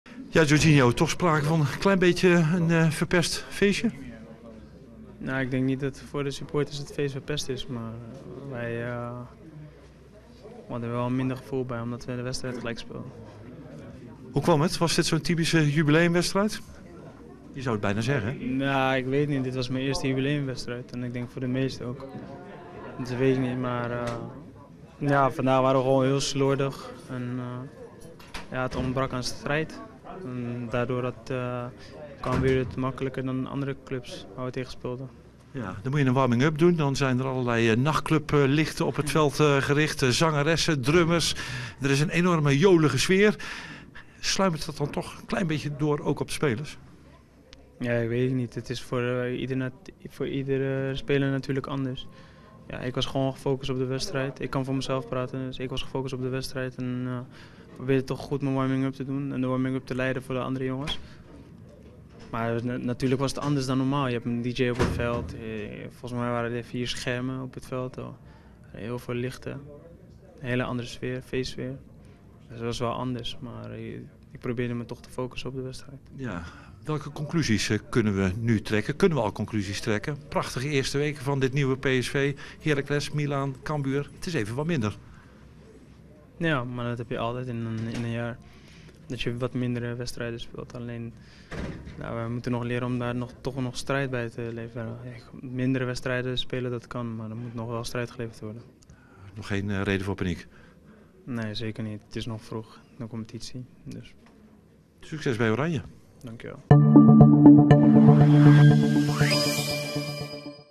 PSV'er Georginio Wijnaldum na gelijkspel tegen SC Cambuur
Het jubileumfeest in het Philips Stadion kreeg zaterdagavond een bittere nasmaak. PSV speelde op de avond van zijn 100-jarig bestaan doelpuntloos gelijk tegen een sterk SC Cambuur.